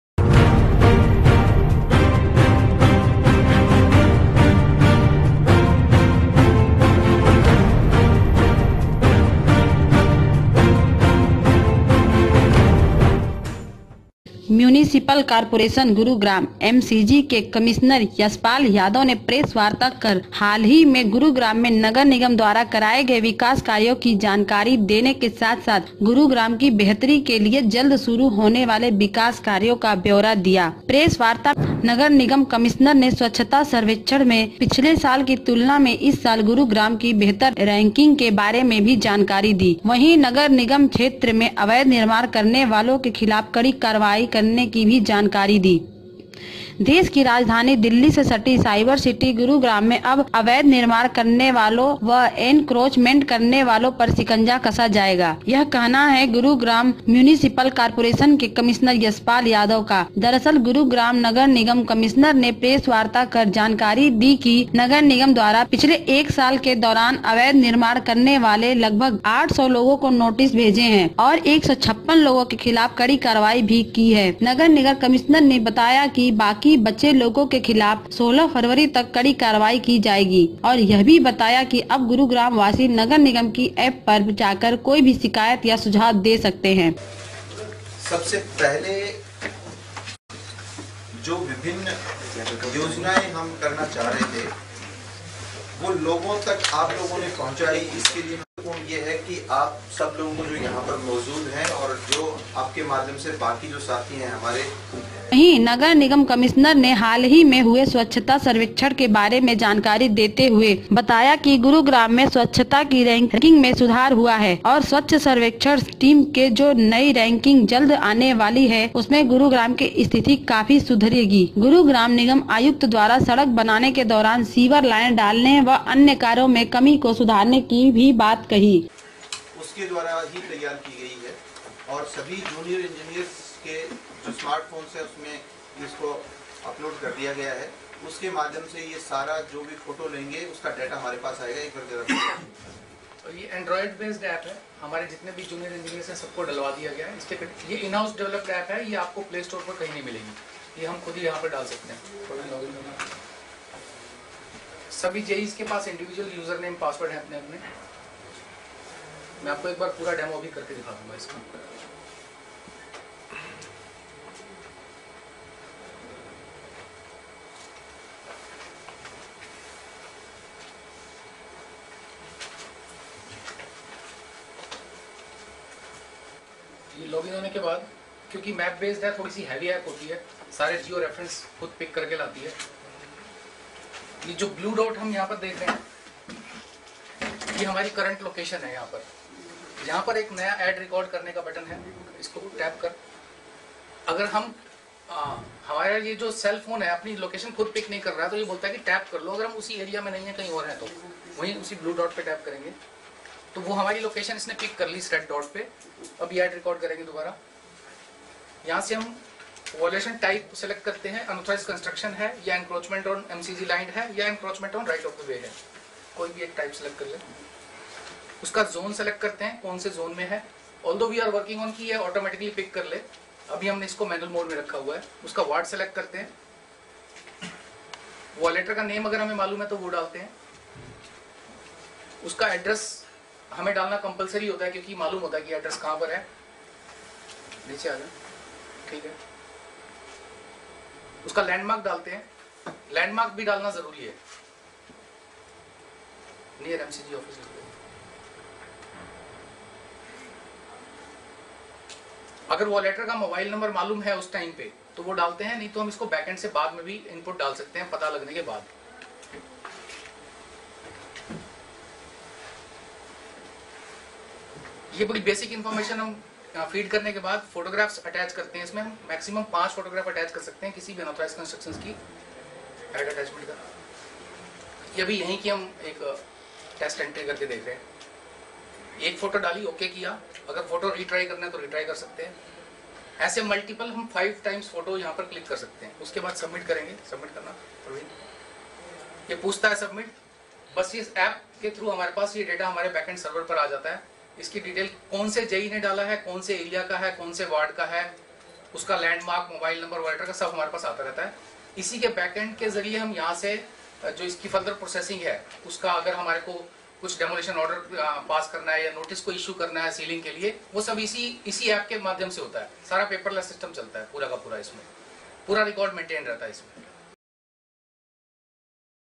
अवैध निर्माण व् एनक्रोचमेंट करने वालो पर कसेगा शिकंजा
गुरुग्राम : नगर निगम कमिश्नर की प्रेस वार्ता
नगर निगम द्वारा कराए गए विकास कार्यों का दिया ब्यौरा
गुरुग्राम की बेहतरी के लिए जल्द शुरू होने वाले विकास कार्यों की रूपरेखा की भी दी जानकारी
स्वच्छता अभियान में गुरुग्राम की बढी हुई रैंकिंग की दी जानकारी
पिछले एक साल में 800 लोगो को नोटिस किये जारी
156 बिल्डिंग्स की गई सील
16 फरवरी तक बाकी बचे केसों के खिलाफ भी की जाएगी कड़ी कार्रवाई
गुरुग्राम नगर निगम की एप पर आम नागरिक दे सकता है शिकायत या सुझाव